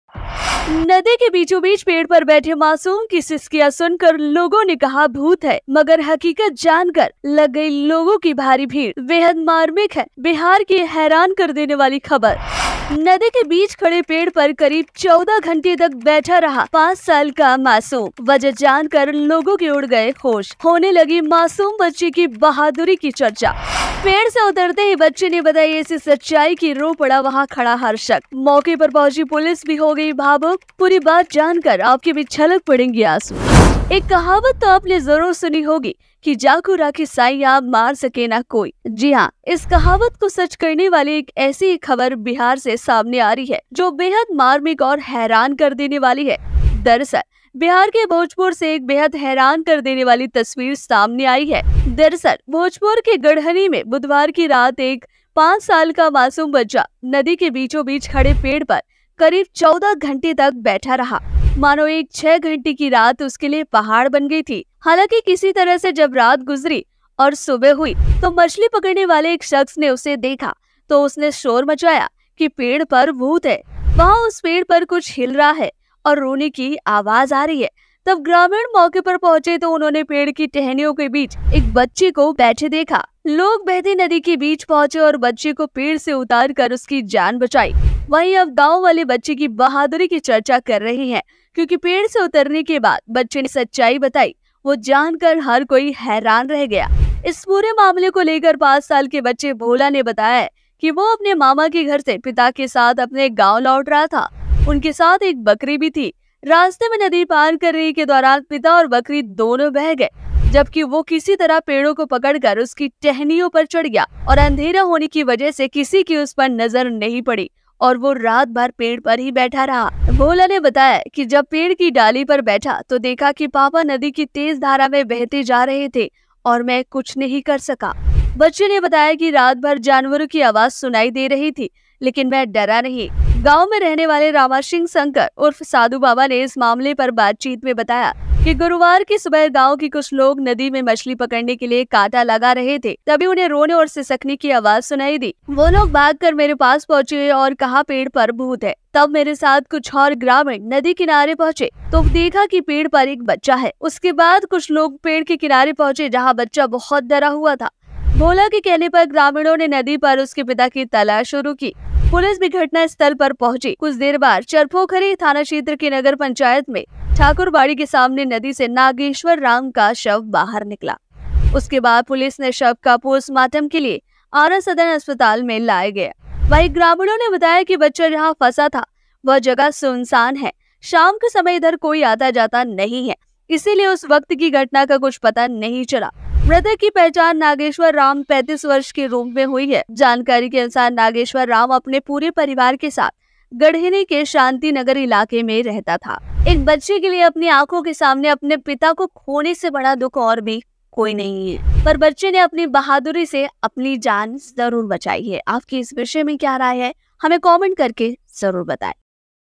bihar || नदी के बीचो-बीच पेड़ पर बैठा रहा मासूम, लोगों ने कहा भूत है... हकीकत जानकर लग गई भीड़
हमारे मुद्दे-

*सनातन संस्कृति
*क्षेत्रीय मुद्दे
*जनजागरूकता अभियान
*जनता की राय
*आदि